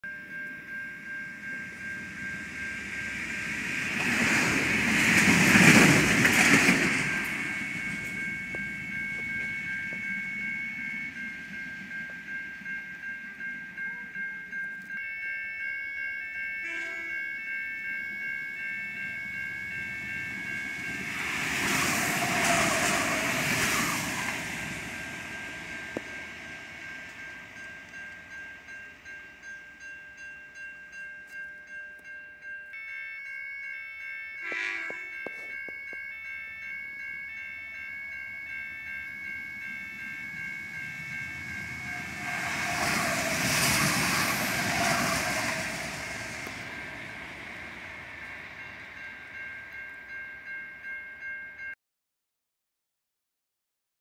Railfanning around The site of Chief st Croydon SA (+13)
3 Comeng Metro trains on the broad gauge railway